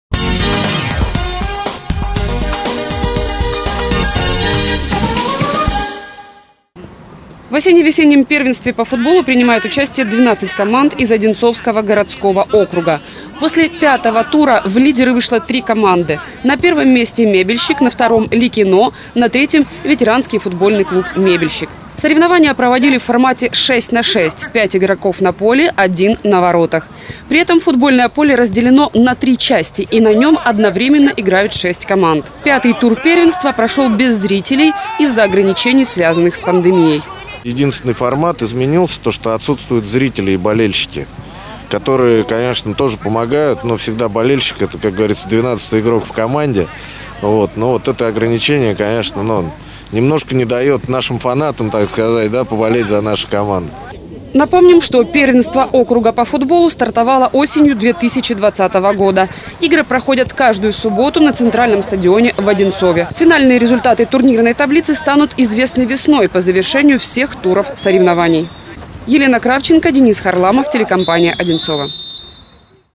Первенство по футболу
На центральном стадионе в Одинцове состоялся пятый тур осенне-весеннего первенства по футболу среди команд городского округа. Игры проходили в формате 6 на 6.